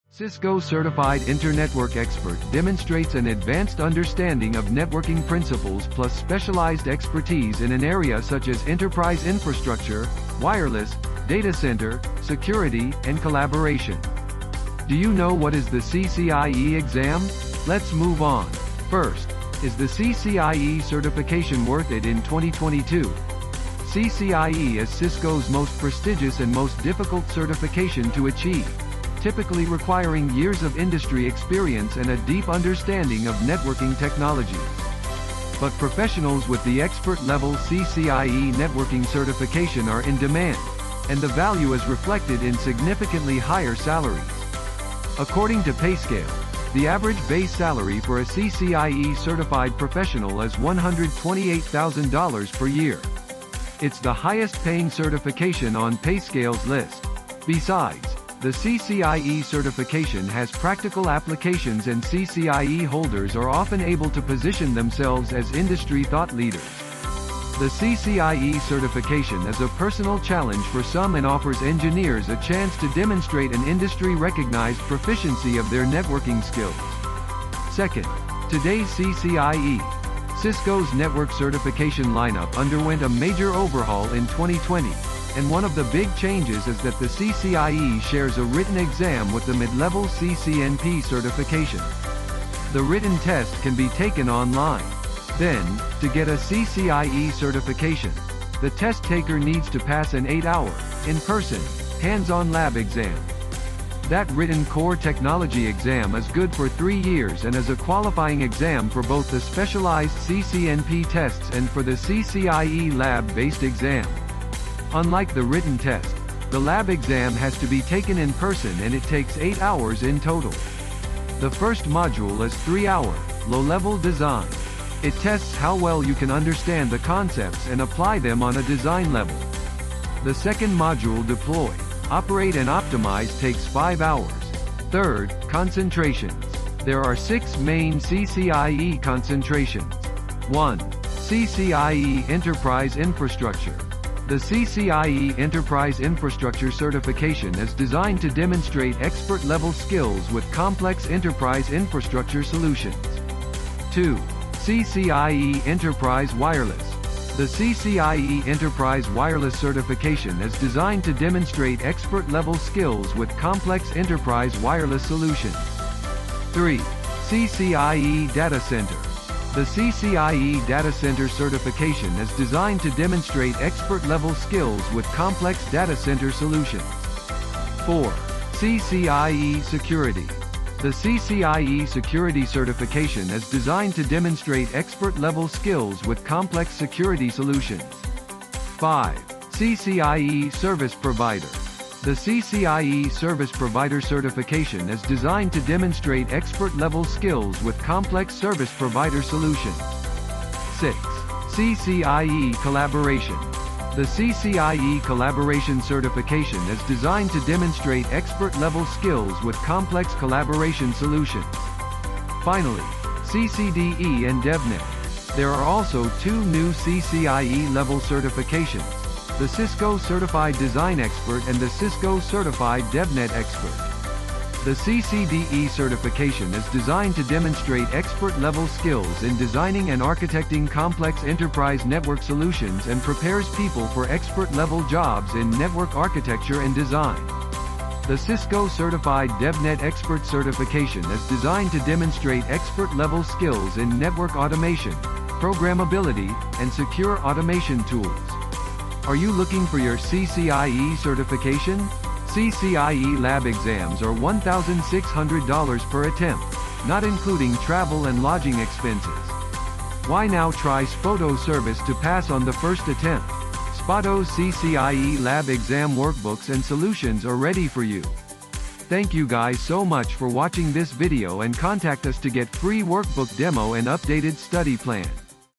What is the CCIE Exam Like? | Is the CCIE Worth It in #2022? | Cisco CCIE certification explained
Cisco Certified Internetwork Expert demonstrates an advanced understanding of networking principles plus specialized expertise in an area such as enterprise infrastructure, wireless, data center, security, and collaboration. Do you know what is the CCIE exam? Let's move on.